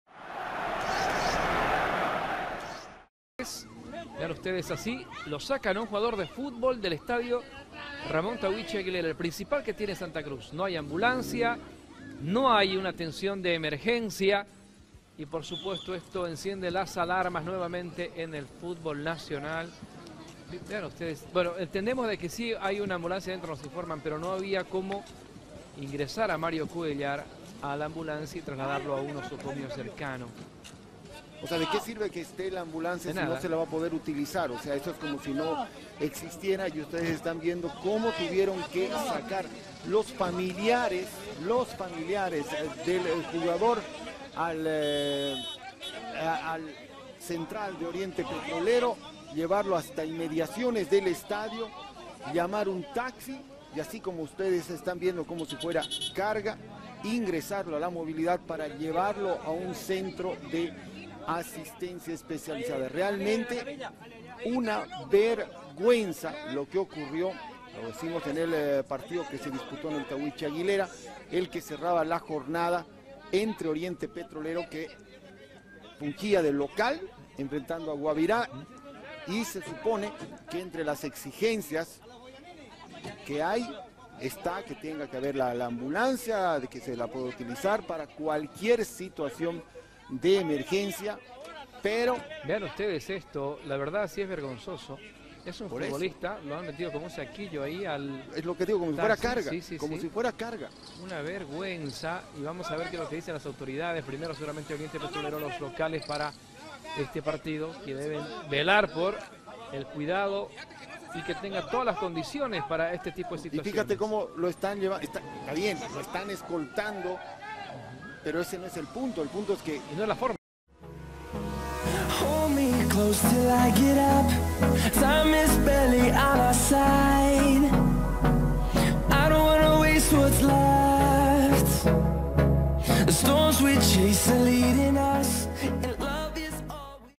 Se ROMPE el tobillo y le llevan al hospital en TAXI con el maletero abierto ⚽ REVIEW ⚽ 2019
🔸 Tremenda la situación vivida en Bolivia. Mario Cuellar, defensa de Oriente Petróleo, se lesionó el tobillo y tuvo que ser trasladado al hospital de una forma que no esperaba para nada.